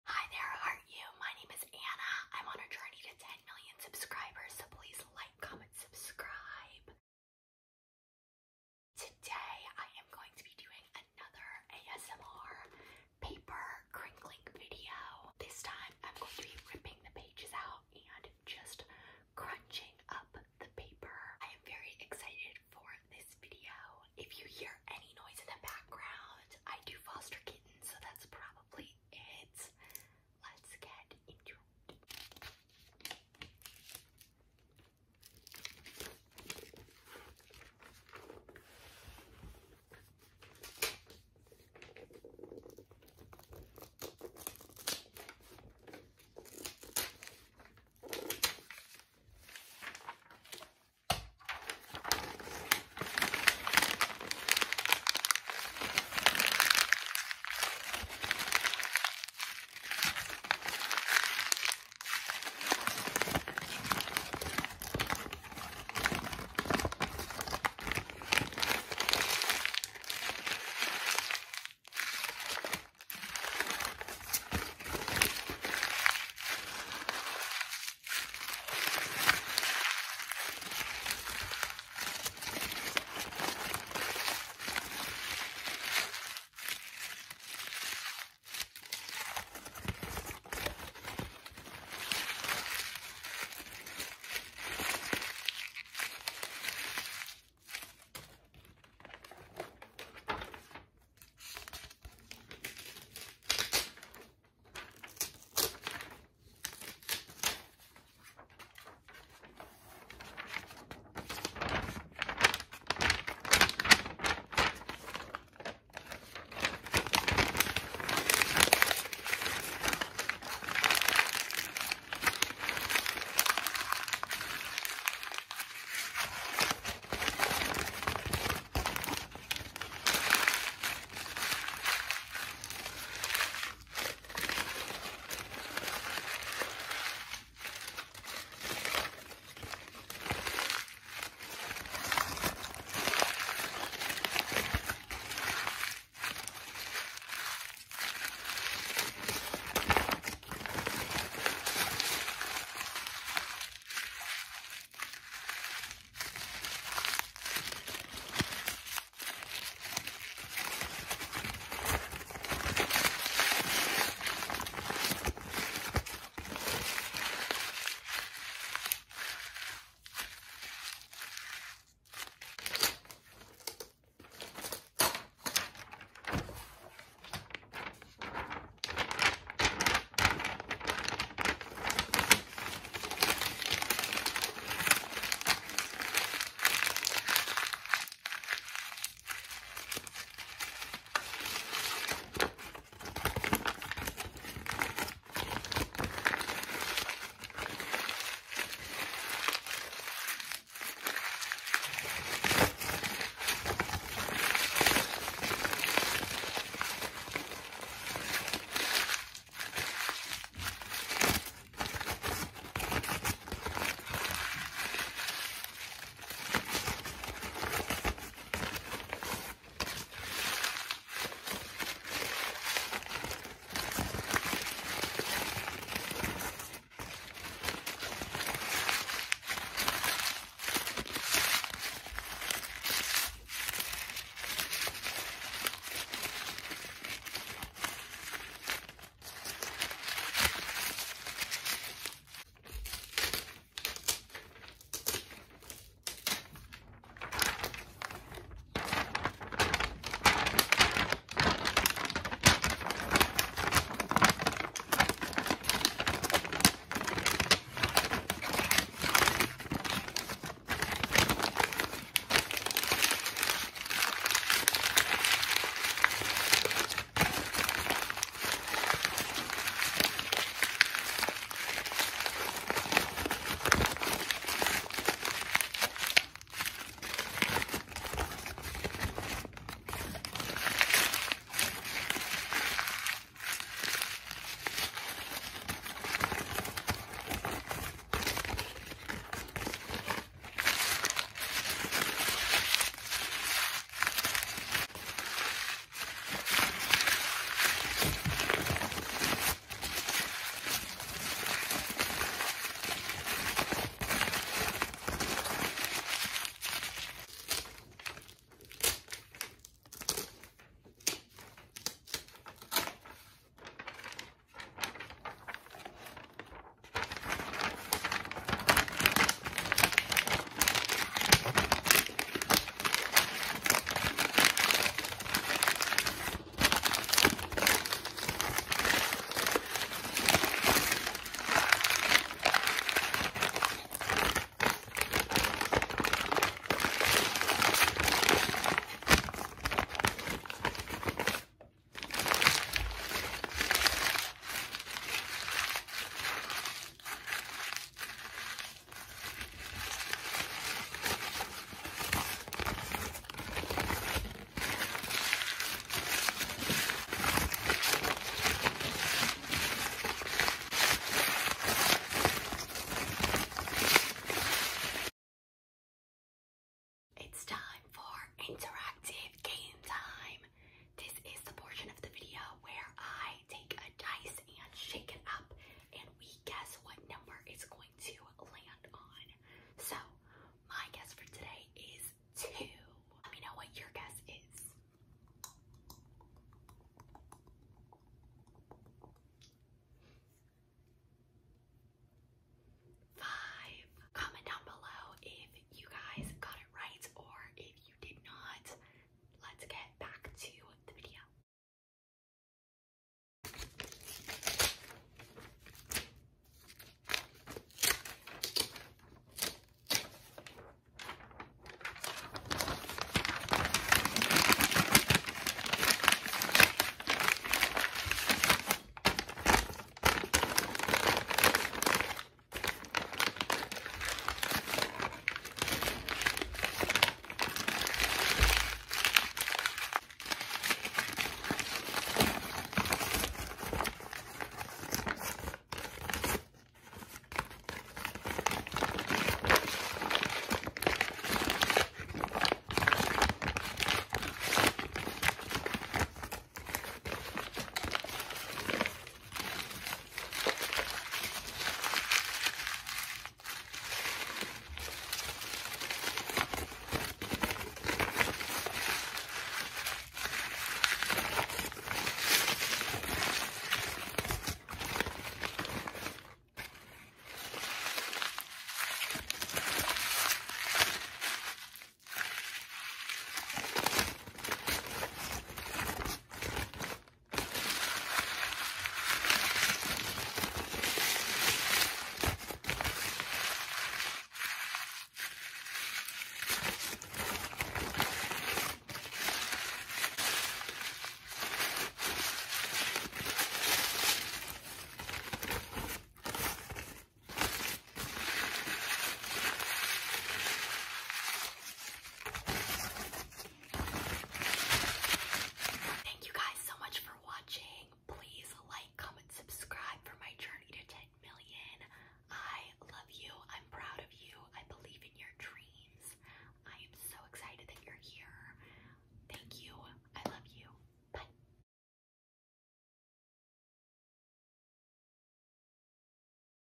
ASMR Paper Crunching - Paper Crinkling, Tearing Pages Out, Game Time
Hey guys,

Welcome to this ASMR paper crinkling video! In this video, I will be creating soothing sounds by turning the pages of my notebook! This video is designed to help you relax and unwind, and to provide you with a calming sensory experience.
As I crunch the pages, you’ll hear the paper crinkle and bend. I’ll provide a variety of sounds for your listening pleasure.
So sit back, relax, and let the crinkling sounds wash over you. Whether you're using this video as a way to fall asleep, or simply to de-stress after a long day, I hope it helps you feel more peaceful and at ease.
Thank you for tuning in to this ASMR page turning video.

Thank you for all the support! I am on a journey to 10 MILLION SUBSCRIBERS so please

Colossians 2:7